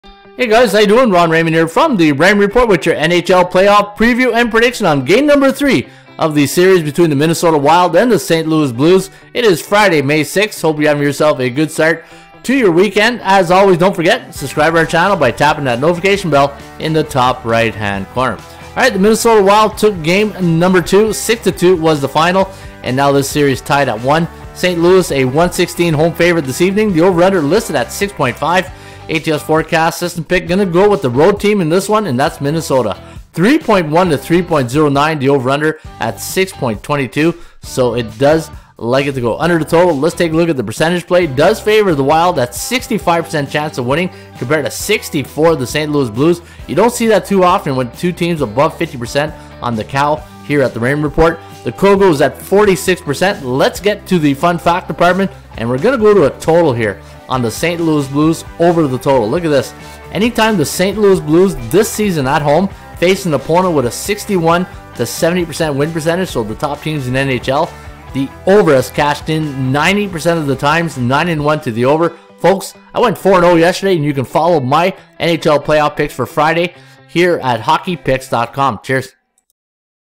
Minnesota Wild vs. St Louis Blues Prediction 5/6/22 - Free NHL Playoff Picks
Free NHL Picks Today: Sports Handicapper Ron Raymond of the Raymond Report has released his free NHL Playoff preview and prediction on today’s NHL Hockey game #3 of the series between the Minnesota Wild vs. St. Louis Blues for Friday, May 6, 2022.